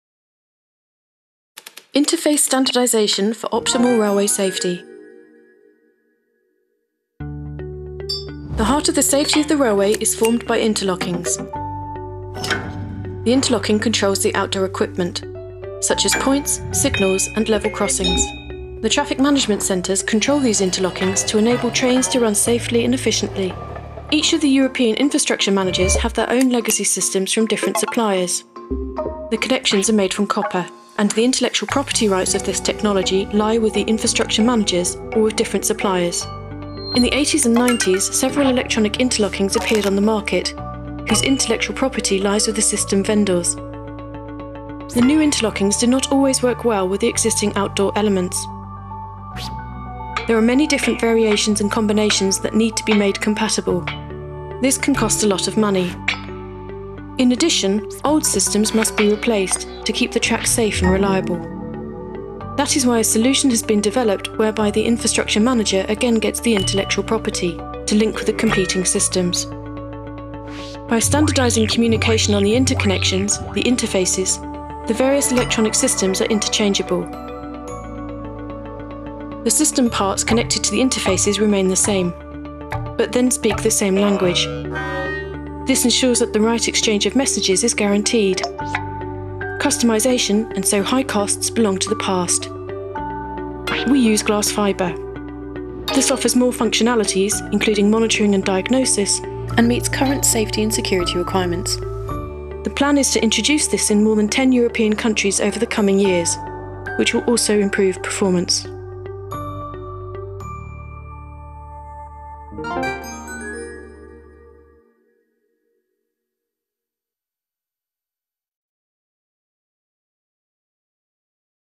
EULYNX Standardization of signalling interfaces (English with subs)
In order to enable future partial replacement of system elements European infrastructure managers feel the need for standardization of the interfaces between these elements. With the EULYNX approach subsystems can be replaced without the need for a complete renewal of the interlocking.